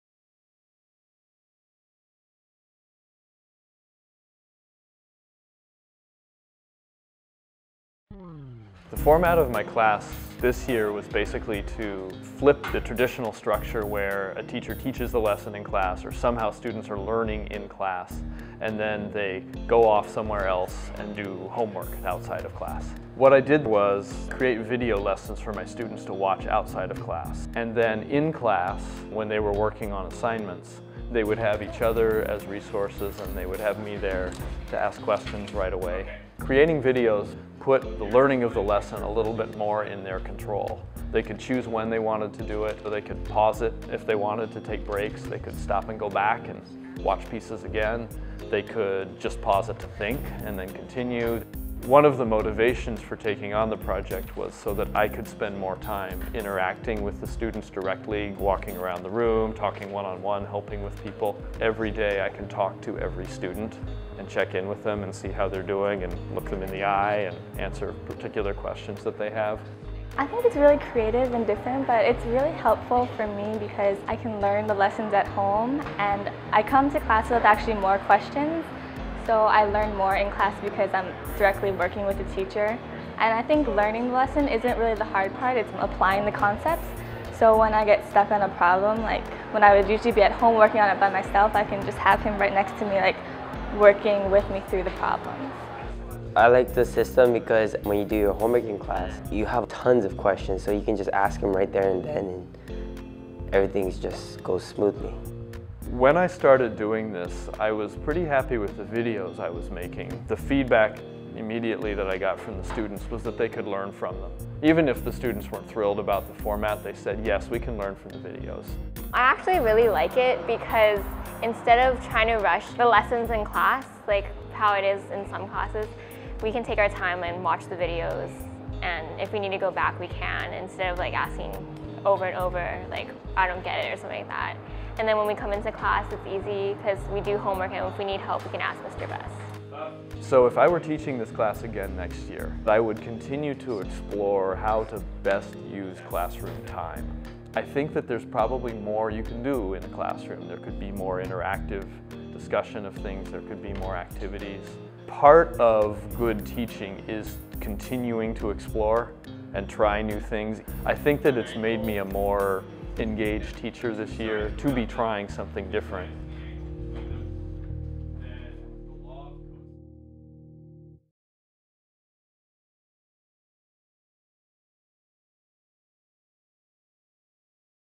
Flipped Class Structure
Academy math class maximizes student-teacher interaction and peer support with video-based lessons at home and homework time in the classroom.